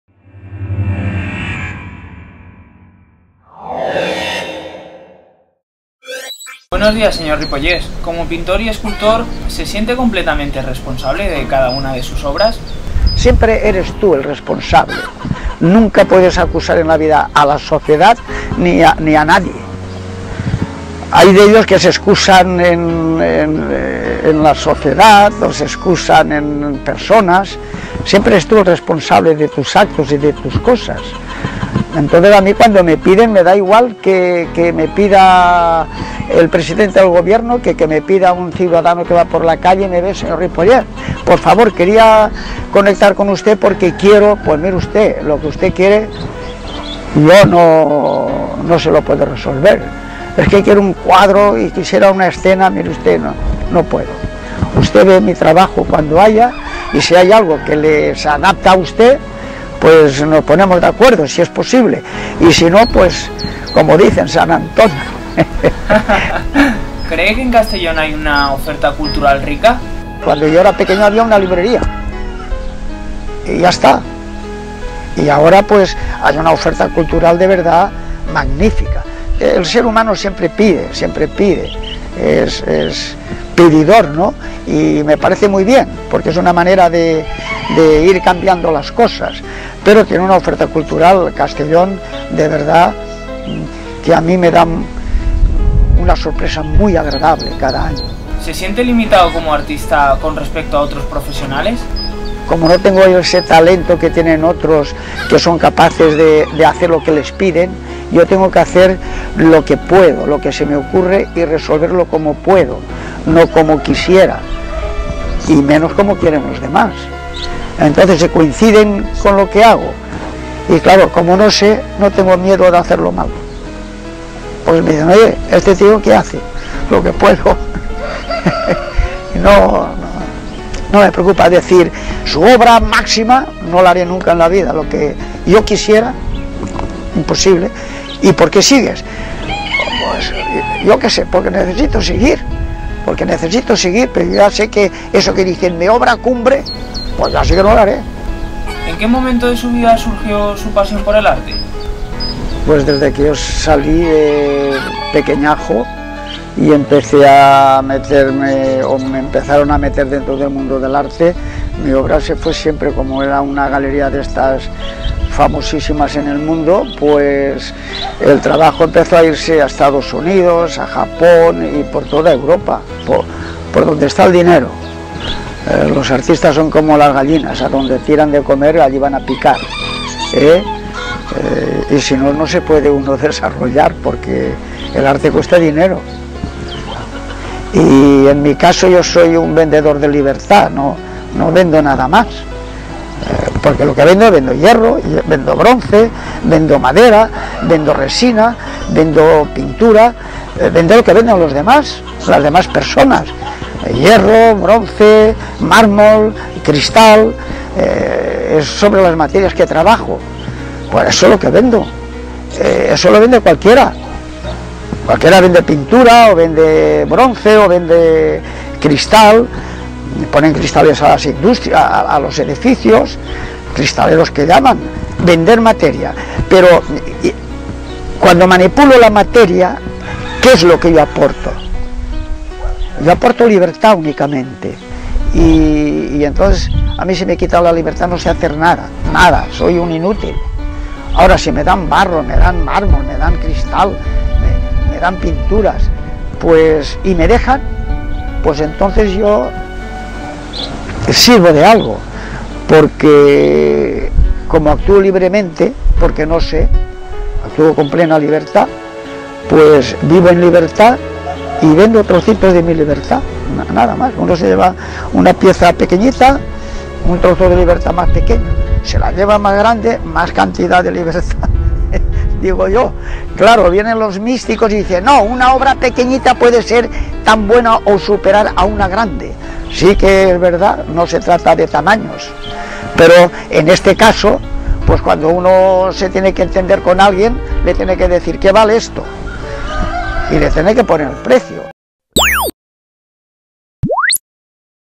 Entrevista a Ripollés en Marketing Audiovisual - La figura de un artista emprendedor
En Marketing Audiovisual tuvimos la oportunidad de entrevistar al artista de Castellón Juan García Ripollés.